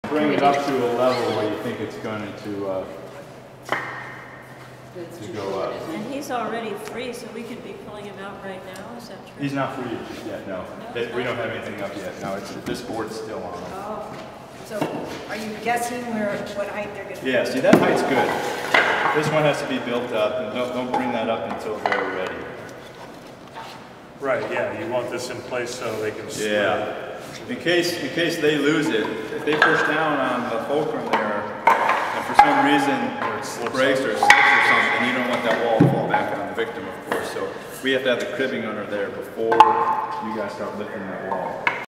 CERT - Session Five - Light Search and Rescue
Partipants learn how to use a fulcrum and cribbing to extricate a victim from a collapsed wall